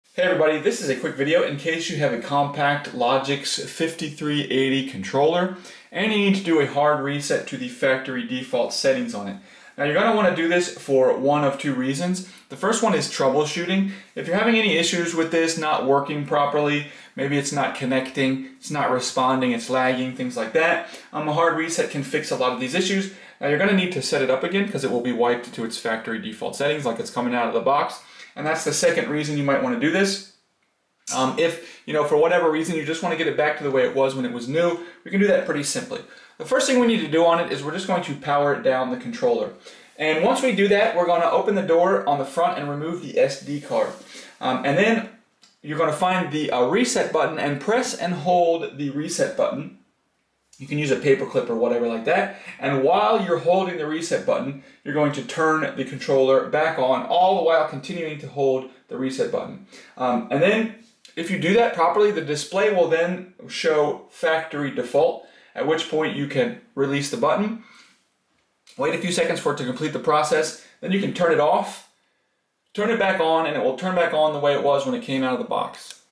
Factory Reset CompactLogix 5380 Controller
How to troubleshoot or setup starting with a hard reset a CompactLogix 5380 Controller to factory default settings if it is not working or not connecting or if it is frozen or not responding, the factory reset is a good troubleshooting step. You can also restore the factory settings to setup your device as new. Found in the CompactLogix 5380 Controller user manual.